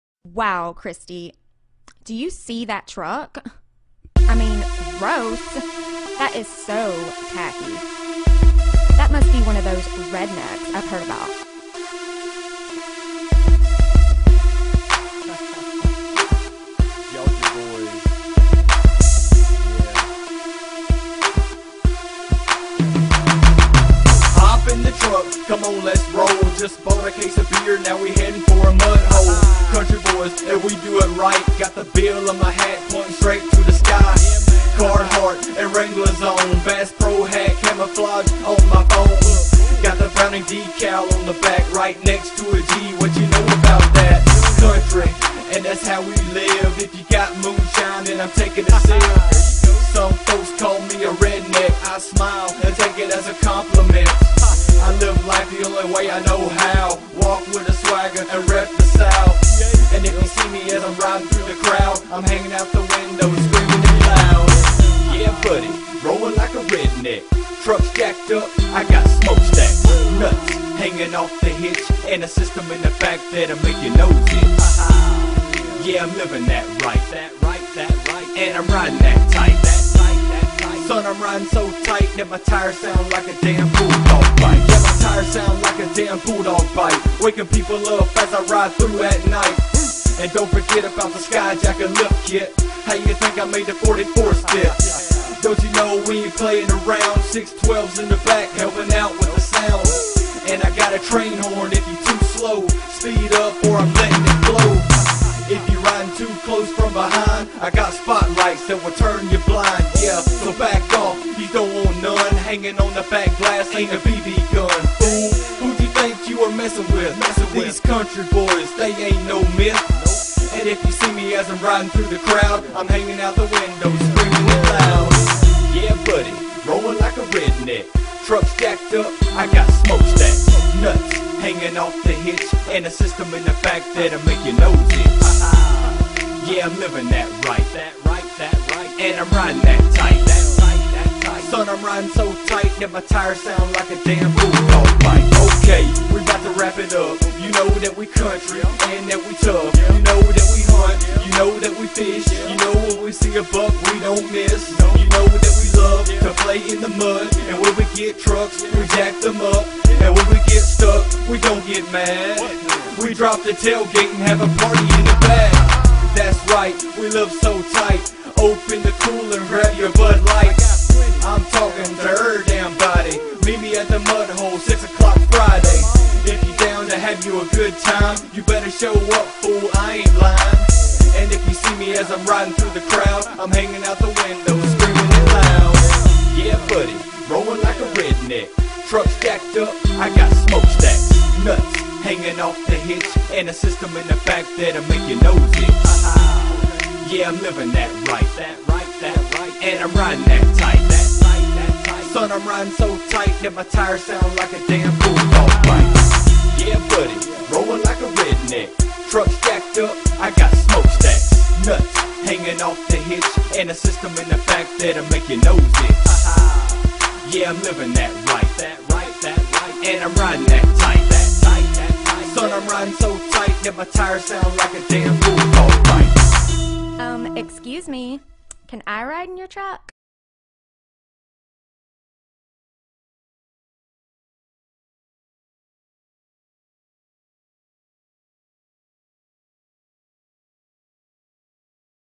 jeep cherokee rollen like a redneck
losi high roller brushless 7.5 turn motor and castle side winder esc